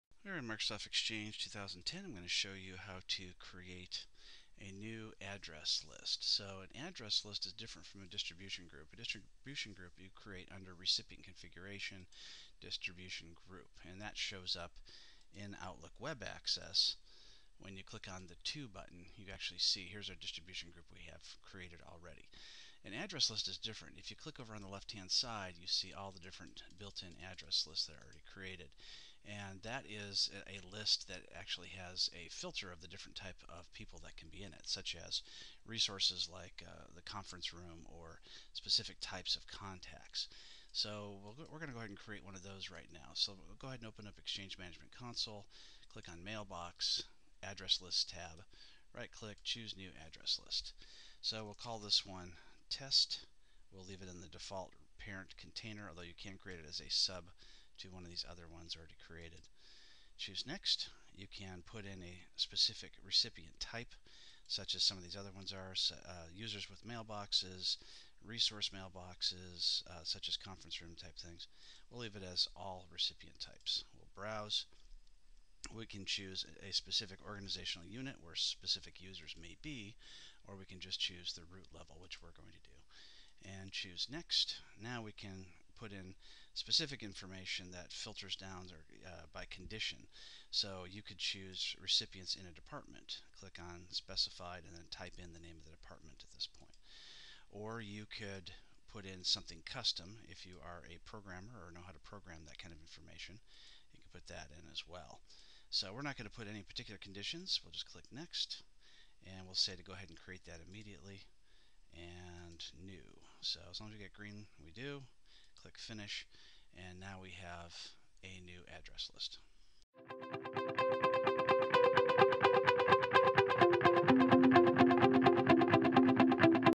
How and why to create an address list in Microsoft Exchange 2010
Author and talk show host Robert McMillen explains how and why to create an address list in Microsoft Exchange 2010.